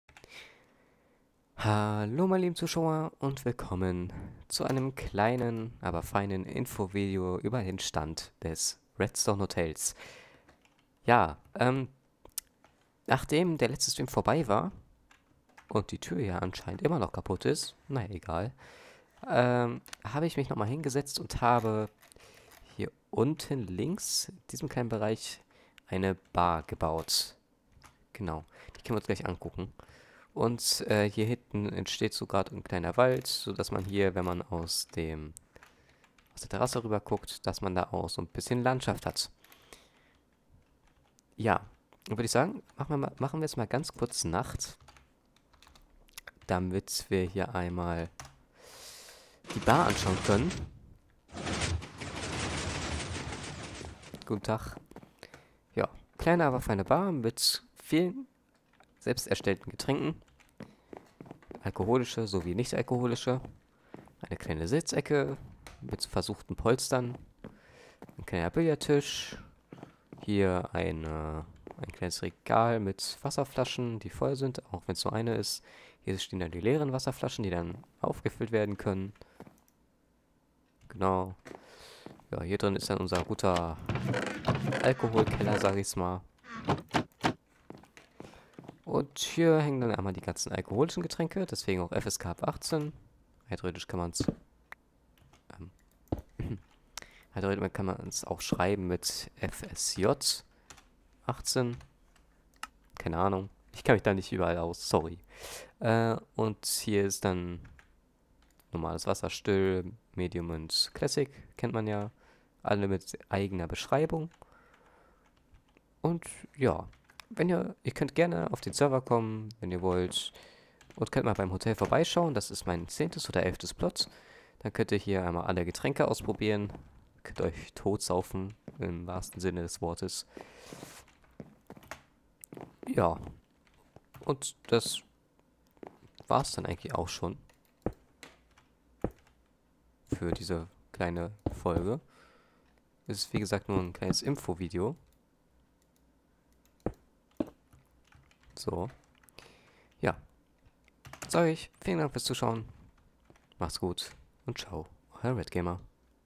Info RedstoneHotel
Das Redstonehotel wurde ja im letzten Stream fast fertig gestellt
Ich habe mich dann nochmal drangesetzt und eine kleine bar eingebaut ;)